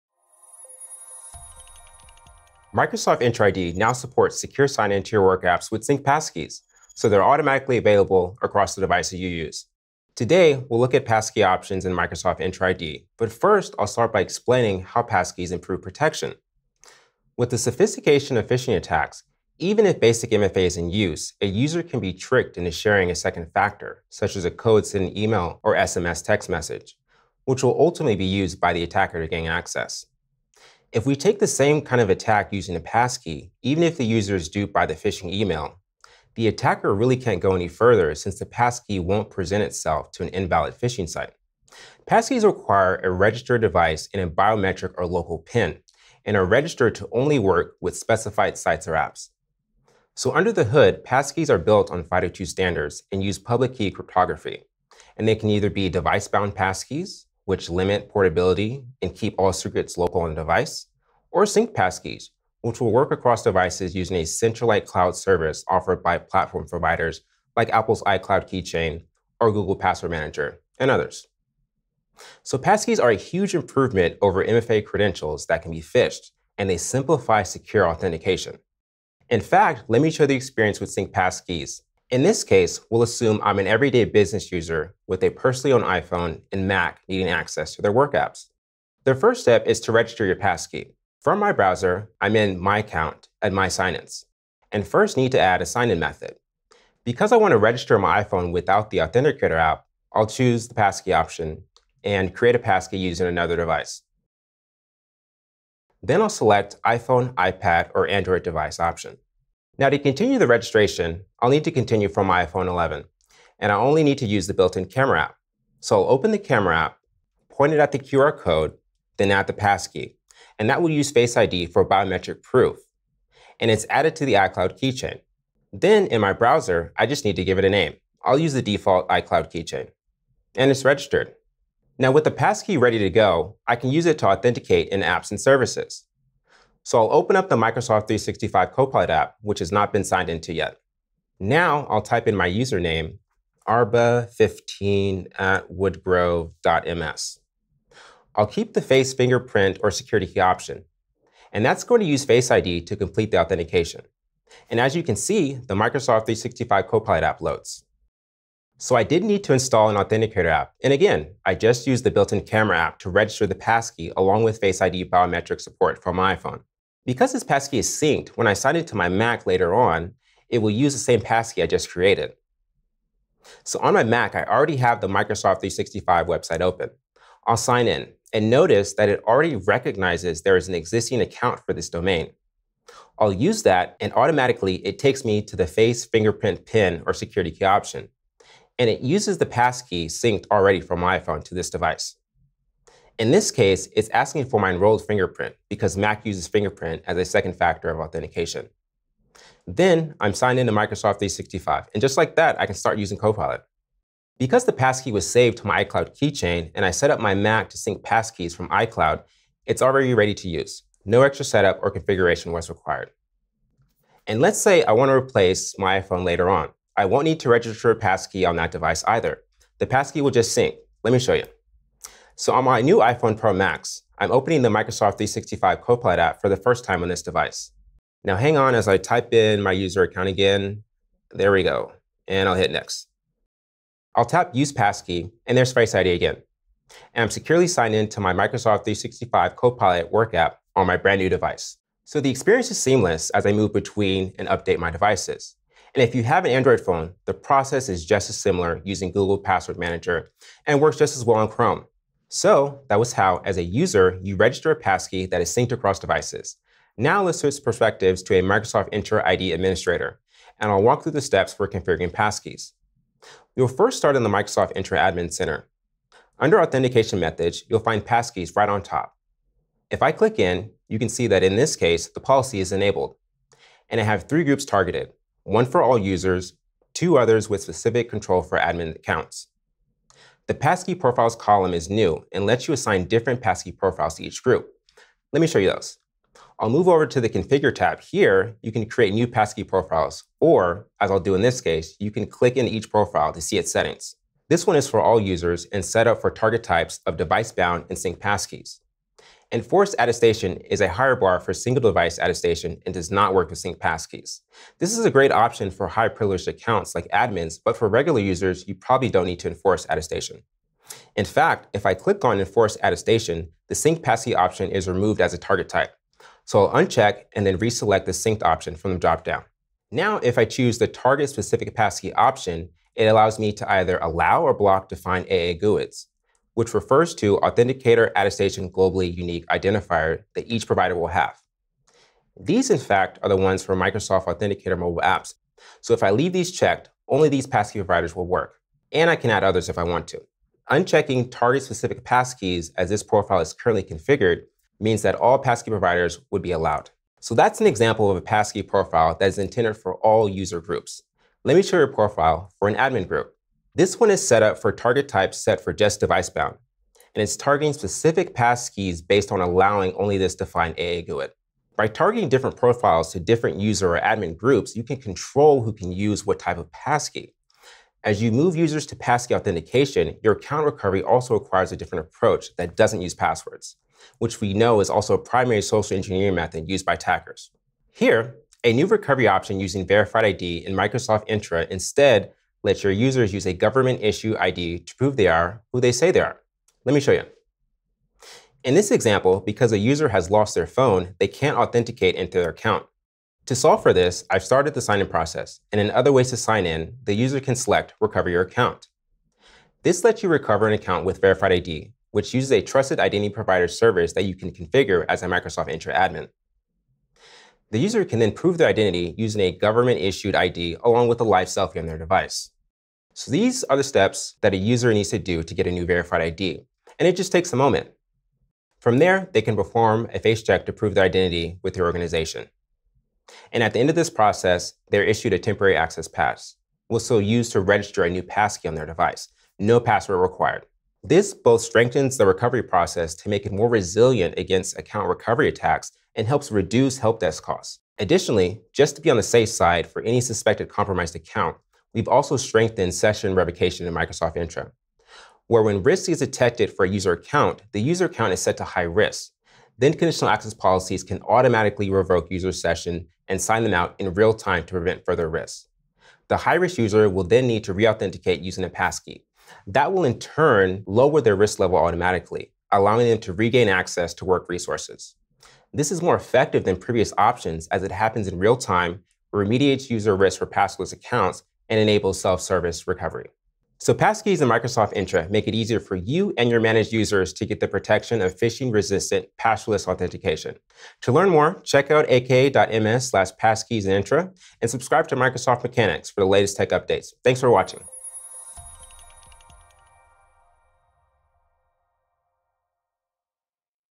Synced Passkeys in Microsoft Entra for Phishing-resistant MFA
Secure sign-in across all your devices without relying on passwords or extra apps using passkeys in Microsoft Entra. Register, sync, and use passkeys with just your device’s camera and biometrics, making authentication seamless, fast, and phishing-resistant. As an admin, control who uses which passkey type, streamline recovery with Verified ID, and automatically remediate risk in real time.

Jarred Boone, Identity Security Senior Product Manager, shows how users can access work apps safely, confidently, and efficiently while reducing help desk overhead.

► QUICK LINKS:
00:00 - Passkeys in Microsoft Entra ID
01:19 - Register your passkey
02:12 - Authenticate into apps & services
03:34 - Sync passkeys on updated devices
04:16 - Configure passkeys as an admin
05:51 - Account recovery
07:18 - Conditional Access policies
07:53 - Wrap up

► Link References